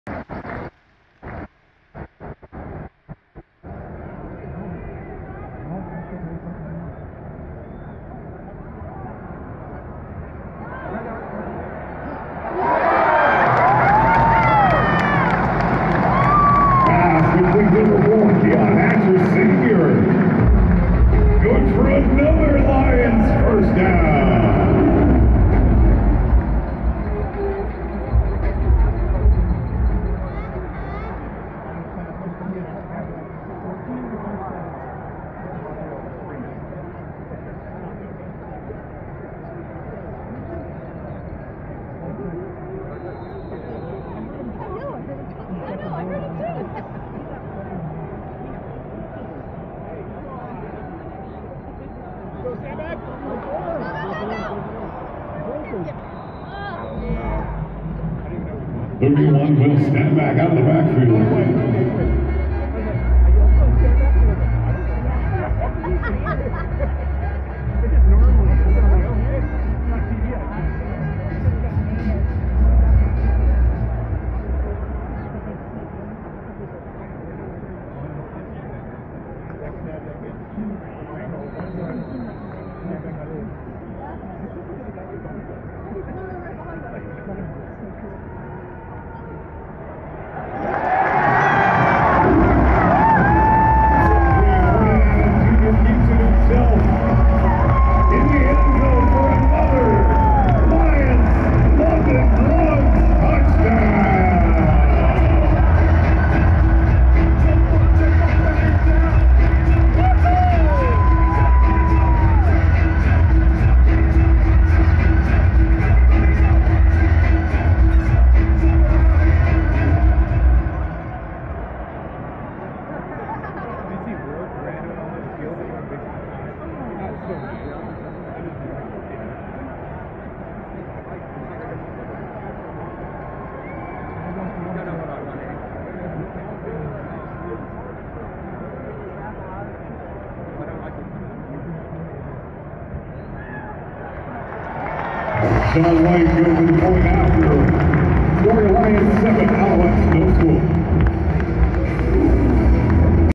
BC Lions Vs Montreal Alouettes October 19th 2024 Vernon Adams Jr Touchdown
BC Lions @cfl